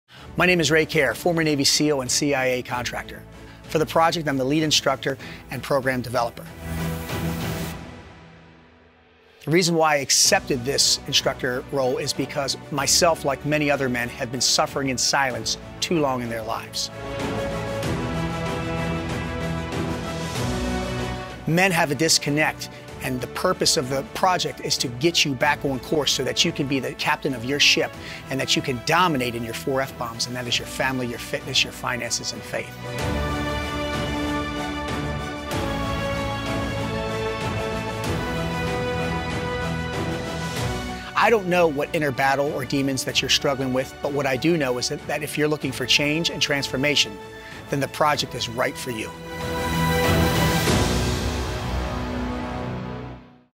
The Project Introduction Video with Ray Care
This program is for men that know they are meant for more, are fed up with mediocrity, and committed to personal excellence looking to take their life to the next level in all areas including Family, Fitness, Finances, and Faith.

Do you consider yourself this type of man at this time in your life?

 All success, All Growth, and Transformation requires some level of Fear and pain, suffering, and sacrifice, and this program requires a significant physical, mental, emotional, and financial commitment -- are you willing to do whatever it takes to reach your true potential at this point in your life?

The Project is a fully immersive 75 Hour Physical, Mental, and Emotional experience for Men where you get the chance to Live, Train, and Learn with a US Marine, a Navy SEAL, Swat operator, MMA fighter, and an Empire builder, learning to kill your inner bitch and unleash the beast, so that you can Level Up and Dominate in your family, fitness, finances, and faith, Forging you into a Modern Day Knight and becoming part of an on-going,  life-long brotherhood of Men of FIRE!